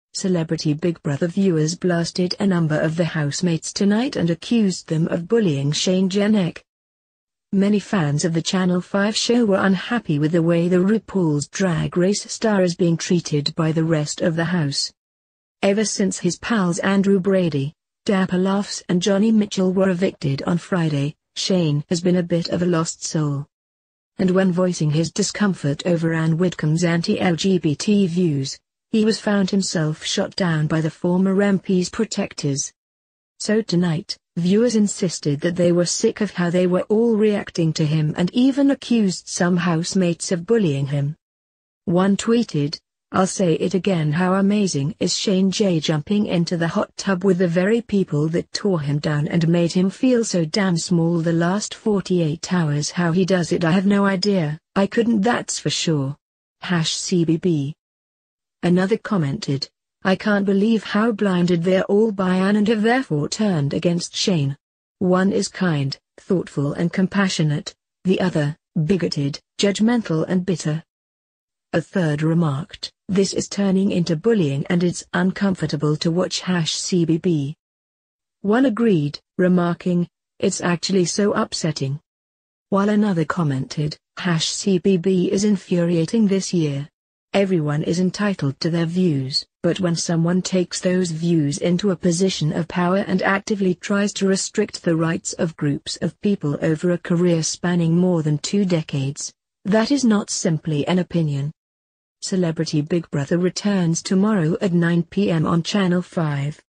'Uncomfortable to watch': Celebrity Big Brother viewers accuse housemates of BULLYING Shane Jenek
Celebrity Big Brother viewers blasted a number of the housemates tonight and accused them of "bullying" Shane Jenek.
Many fans of the Channel 5 show were unhappy with the way the RuPaul's Drag Race star is being treated by the rest of the house.
 Ever since his pals Andrew Brady, Dapper Laughs and Jonny Mitchell were evicted on Friday, Shane has been a bit of a lost soul.